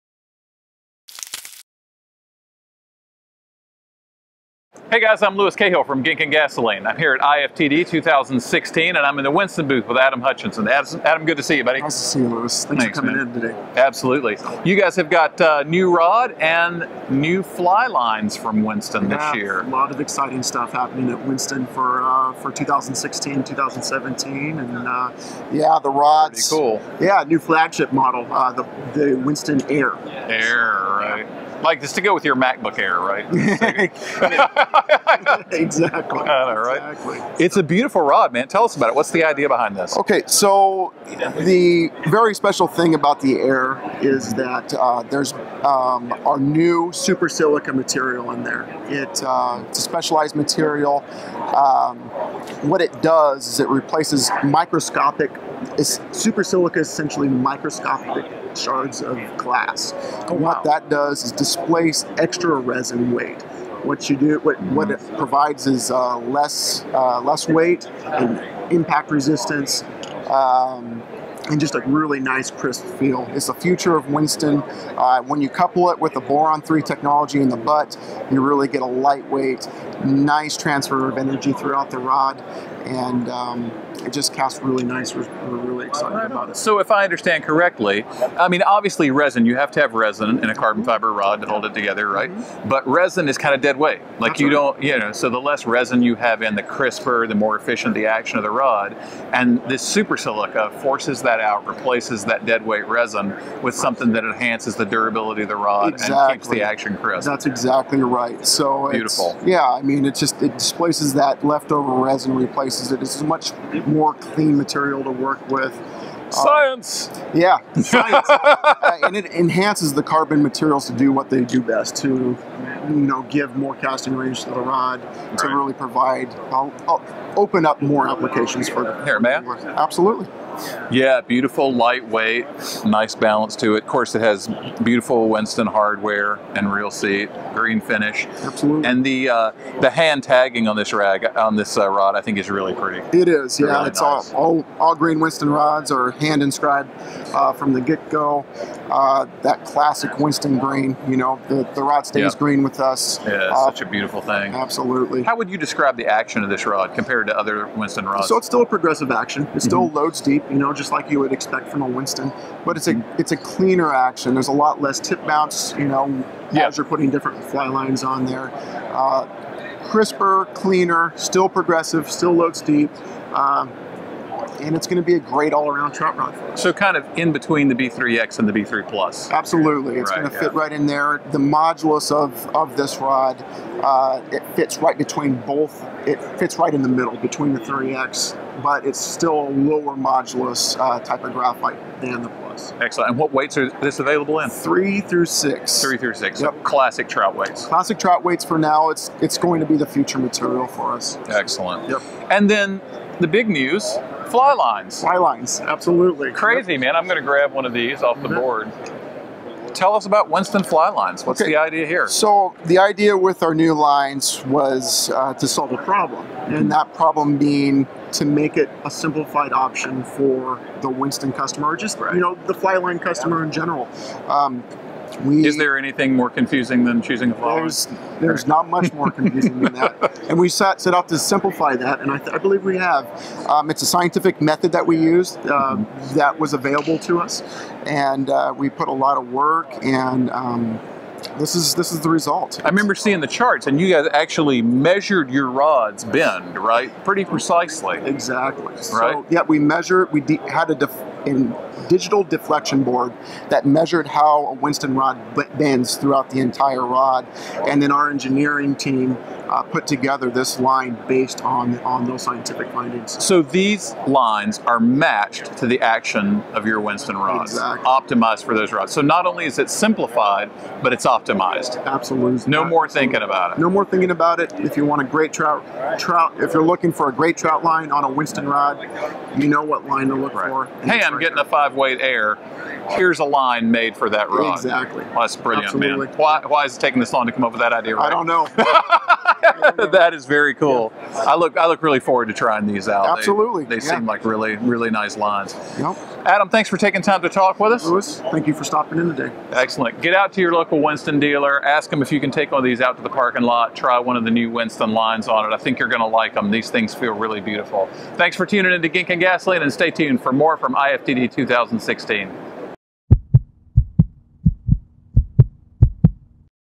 IFTD 2016: Winston Rod Company
The new Winston Air fly rod takes advantage of new high tech resins to make a rod that’s faster and lighter. The action is familiar to the Winston fan, but definitely a step into the future. What’s more, you can buy a Winston fly line matched to your new rod. How cool is that idea? Fly lines made specifically for your new Winston fly rod. An idea so simple, you have to wonder why it hasn’t happened before.